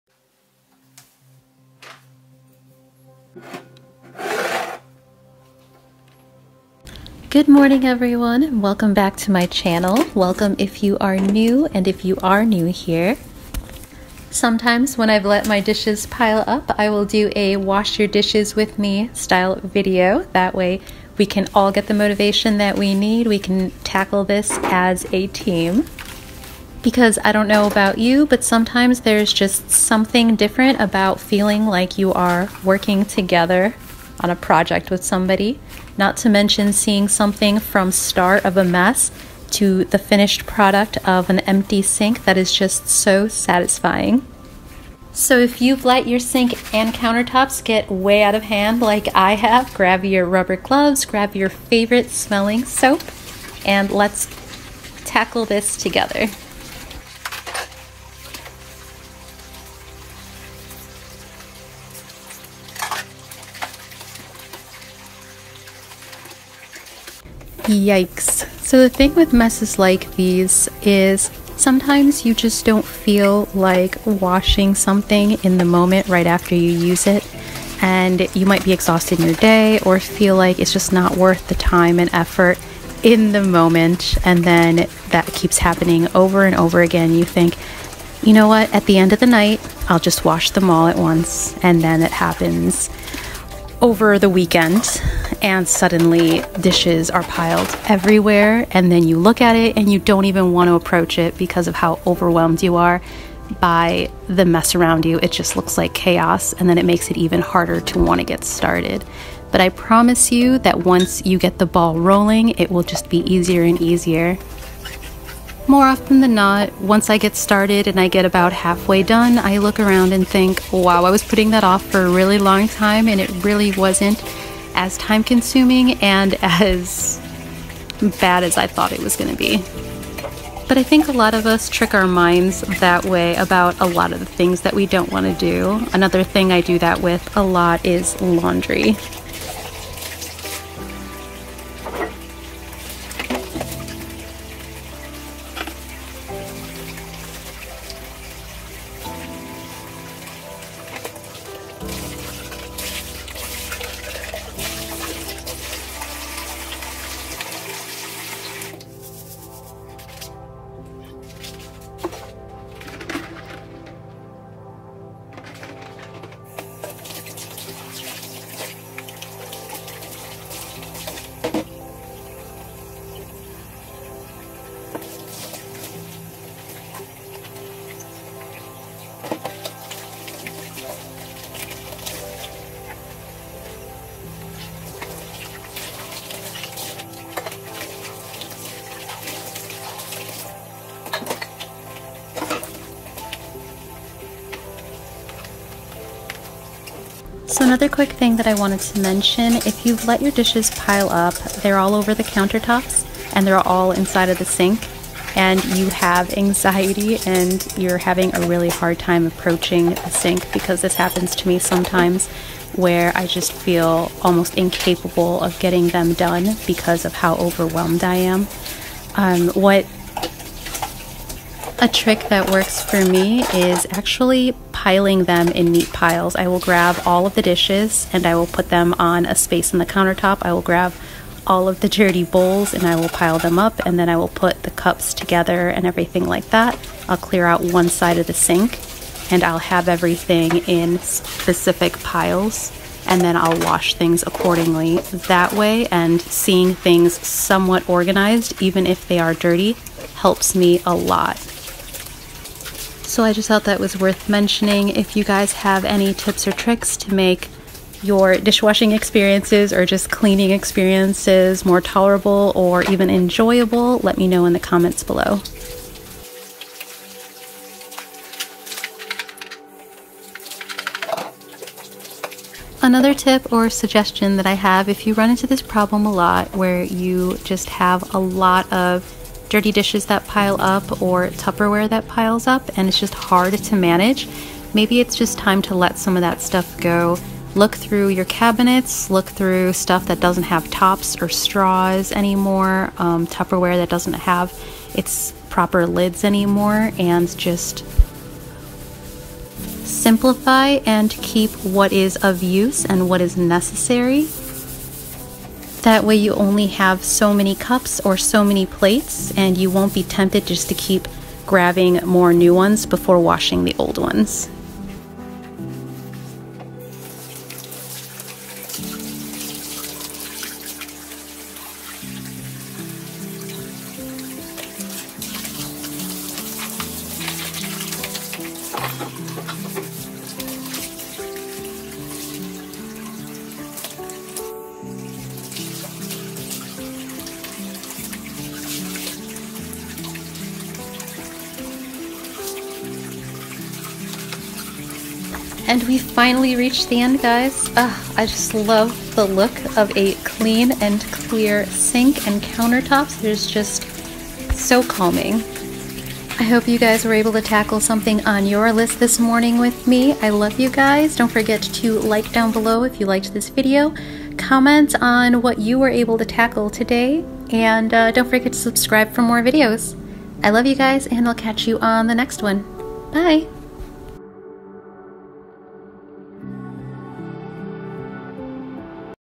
clean with me anxiety stress relief series
Hey Everyone, today’s video is a clean with me with tips on cleaning with anxiety when things pile up. Hope it was helpful and were able to tackle something today!